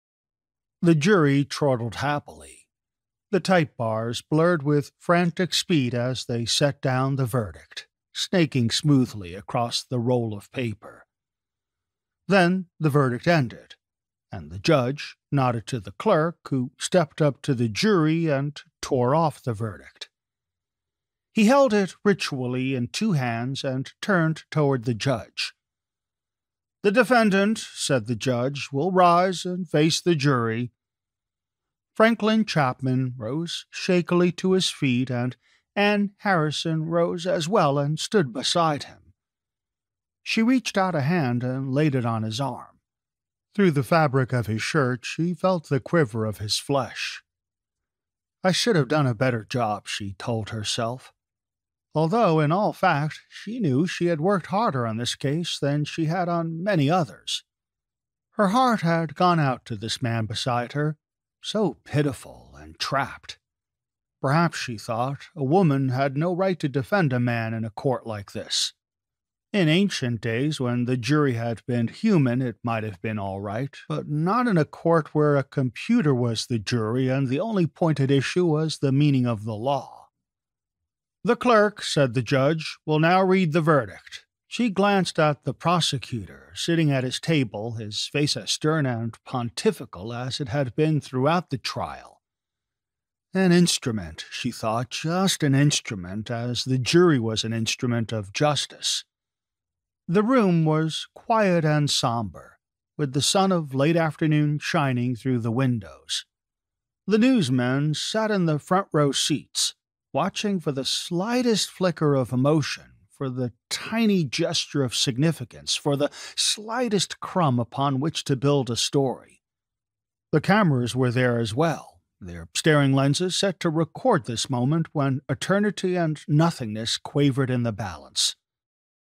Audiobook Sample: Why Call Them Back From Heaven?
A corporation promising immortality hides a sinister secret in this "extremely provocative" sci-fi novel (Judith Merril, author and editor).

Since the dawn of mankind, immortality has been the ultimate reward. But by the year 2148, it requires no act of faith to believe in an afterlife. Forever Center promises to bring people back to a life beyond death. Now everyone spends their lives in poverty, giving all their money to Forever Center to ensure their happiness and comfort in the next eternal life.

Daniel Frost is a key man at Forever Center, but when he accidentally stumbles onto some classified documents, Dan incurs the wrath of an unseen enemy and is framed for a terrible crime. Now, his right to immortality has been revoked and he is a social outcast, condemned to the desperate life of a hunted animal. As a renegade lawyer and a brilliant mathematician attempt to help him, they reveal some shattering information about Forever Center . . . and the essence of life itself.